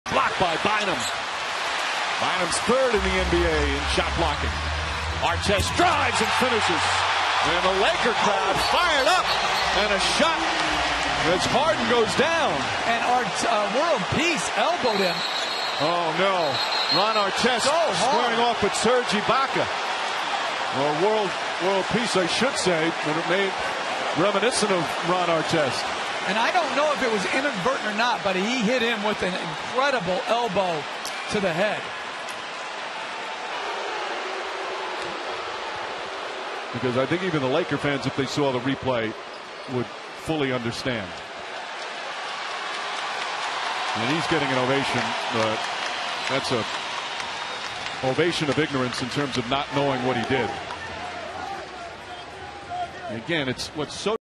NBA World Peace Ejected OKC VS LAL 4/22/2012
Ron Artest or Metta World Peace makes a great dunk, celebrates, elbows James Harden to the floor and is ejected for a flagrant two foul.